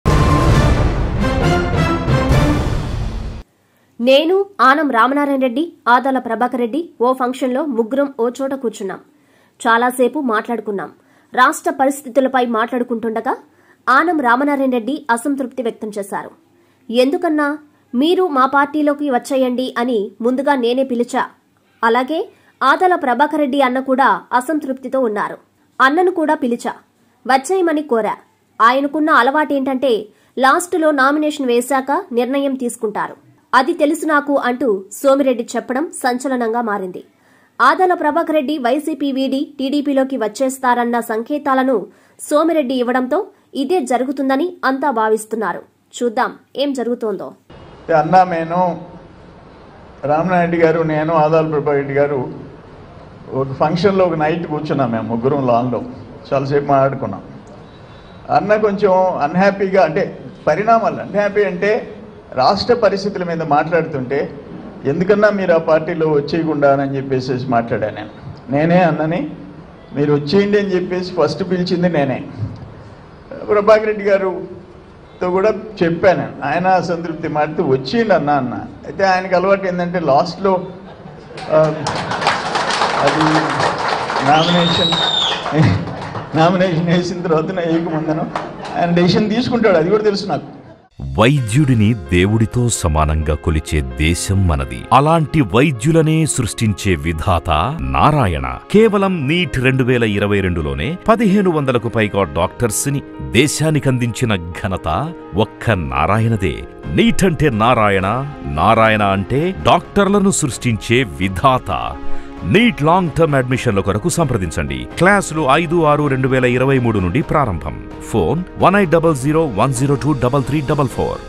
ఆదాల లాస్ట్‌లో టీడీపీలోకి వ‌చ్చేస్తారు..!!
-మాజీ మంత్రి సోమిరెడ్డి సంచ‌ల‌న వ్యాఖ్య‌లు